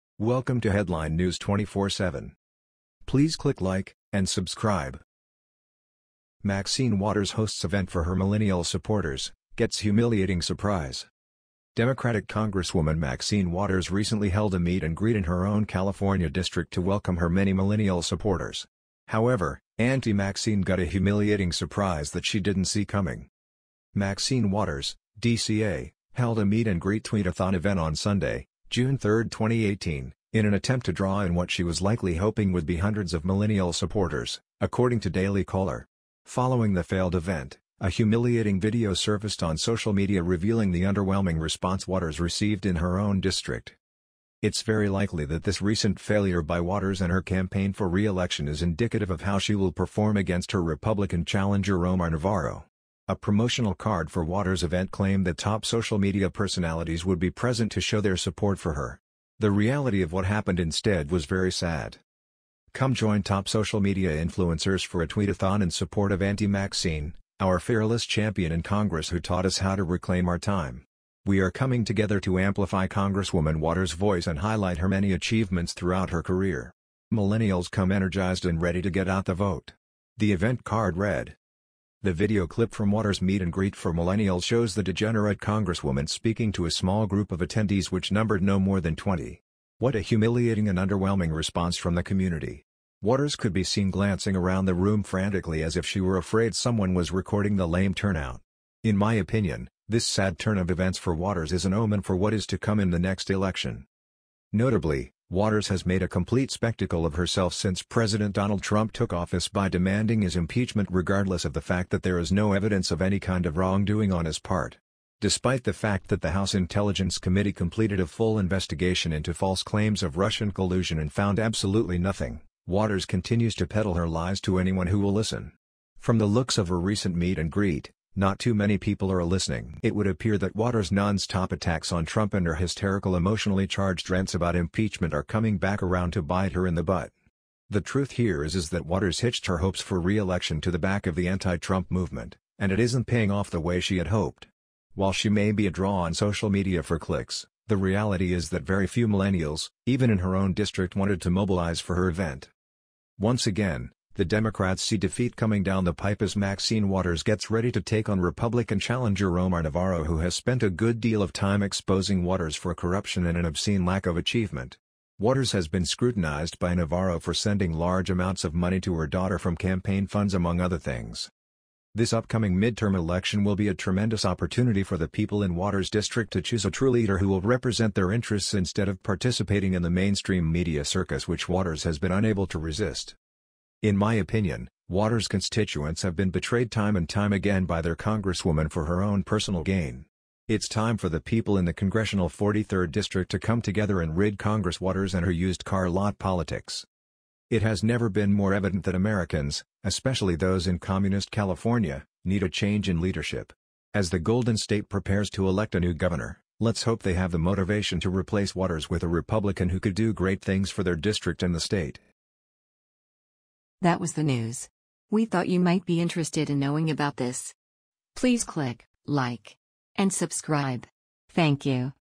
Maxine Waters Hosts Event For Her Millennial Supporters, Gets Humiliating Surprise
The video clip from Waters’ meet and greet for millennials shows the degenerate Congresswoman speaking to a small group of attendees which numbered no more than twenty. What a humiliating and underwhelming response from the community.